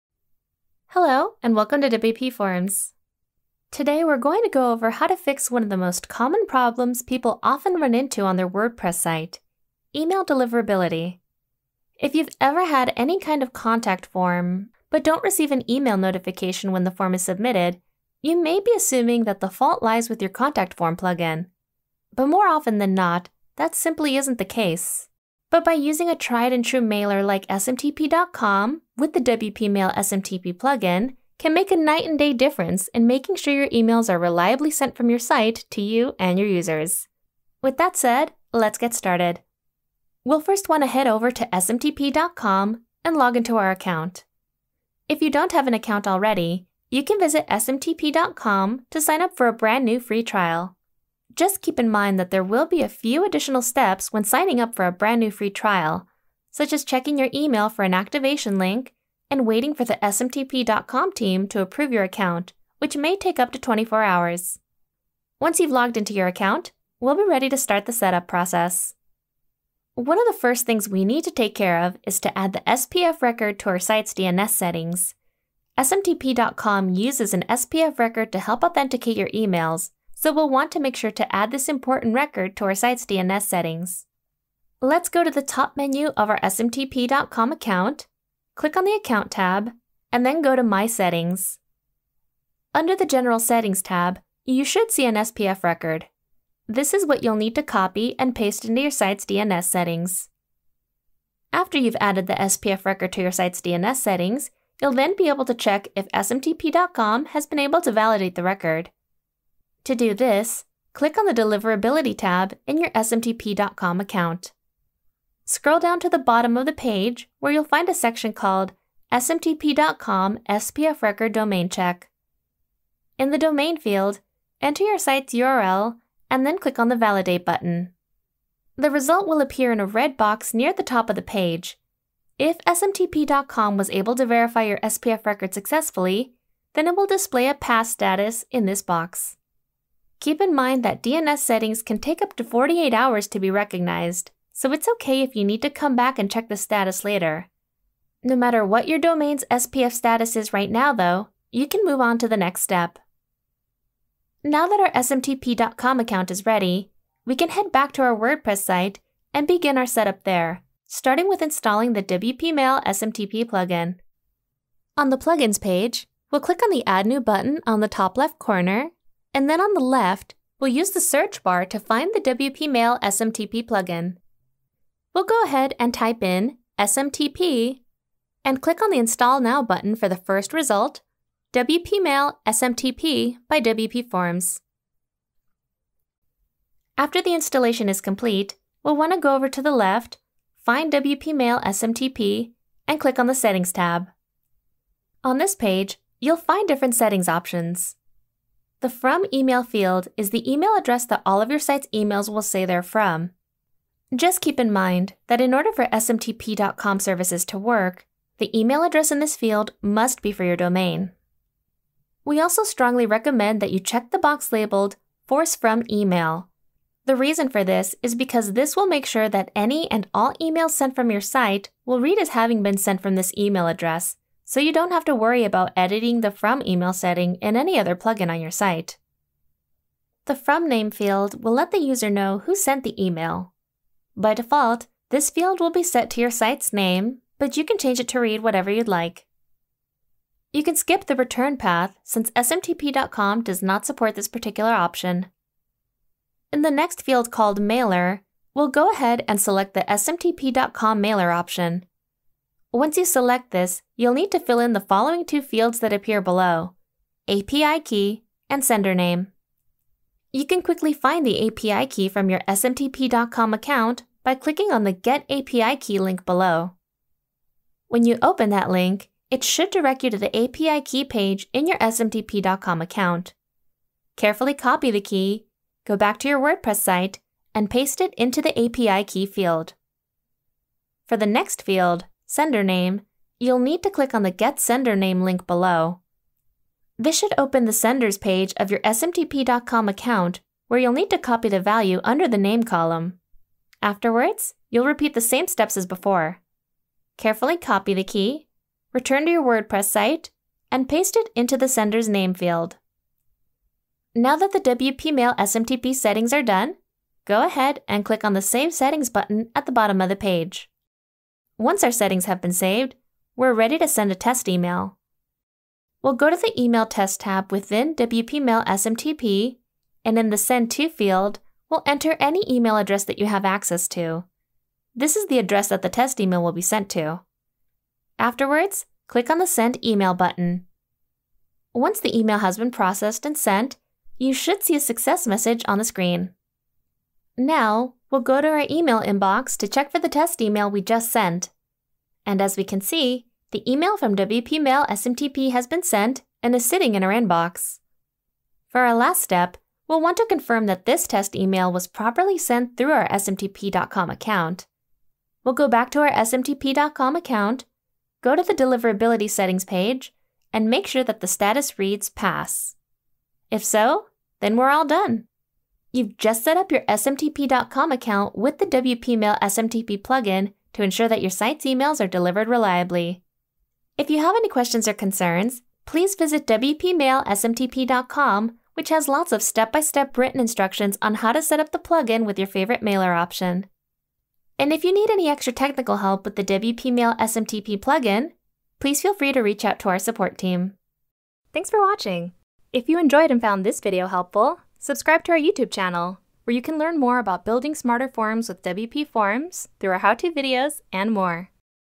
How to Set Up the SMTP.com Mailer in WPMail SMTP (Step by Step Guide!)
Welcome to WPForms!

If you’ve ever had any kind of contact form, registration application or the like on your site but don’t receive an email notification when the form is submitted, you may be assuming that the fault lies with your contact form plugin.

00:00 - Introduction
00:40 - Log Into Your SMTP.com account
01:10 - Adding SPF Record to Your Site's DNS
02:33 - Installing WP Mail SMTP
03:15 - Configuring WP Mail SMTP Settings
05:15 - Sending a Test Email
06:13 - Contact Us!